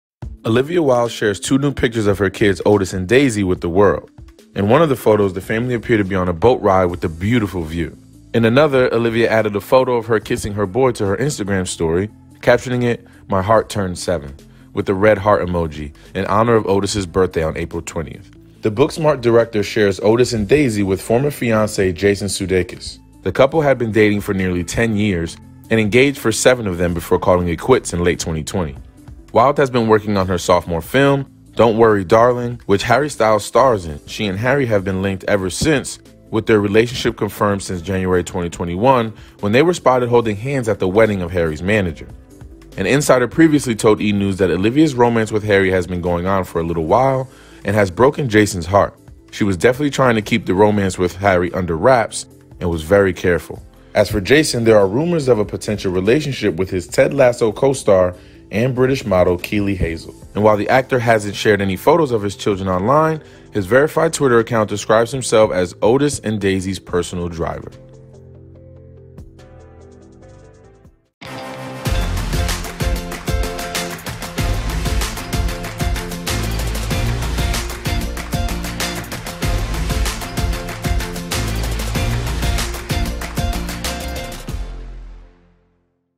Olivia Wilde Shares Rare Pics of Her & Jason Sudeikis' Kids | E! News
The "Booksmart" director, who rarely posts photos of her family, takes to IG to celebrate her eldest son's birthday. See the heartwarming pics!